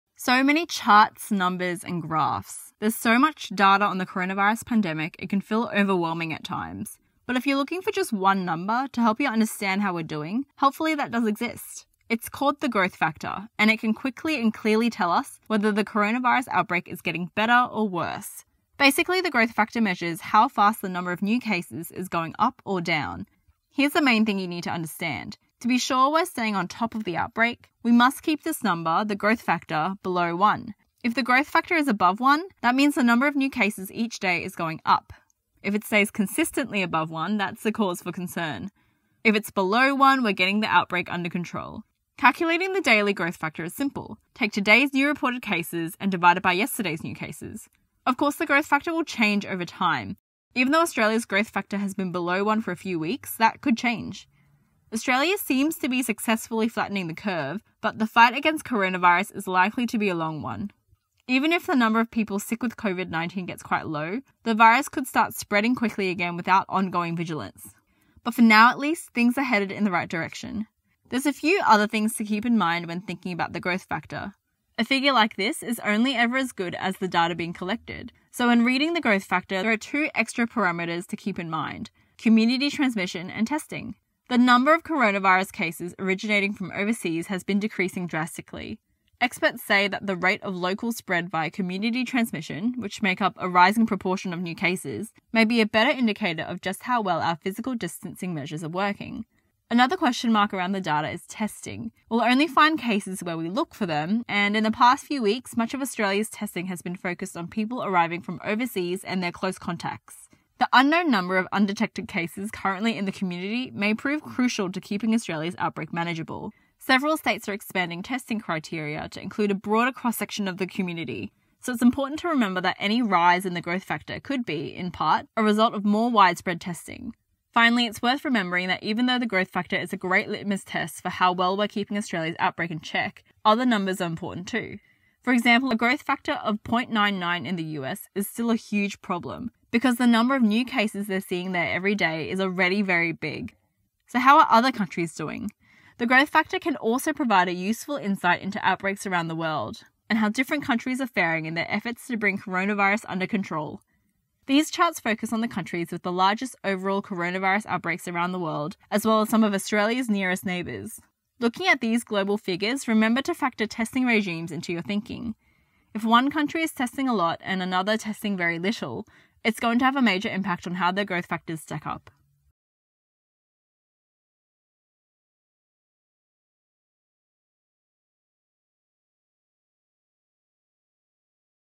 Coronavirus: The one COVID-19 number to watch
There's one number that will tell us if Australia's coronavirus outbreak is getting better or worse.

See up-to-date numbers for Australia, the United States, China and other countries: ab.co/growthfactor

So many charts, numbers and graphs. There’s so much data on the coronavirus pandemic, it can feel overwhelming at times.

But if you’re looking for just one number to help you understand how we’re doing, helpfully that does exist. It's called the growth factor — and it measures how fast the number of new cases is going up or down.

RESOURCES
The one COVID-19 number to watch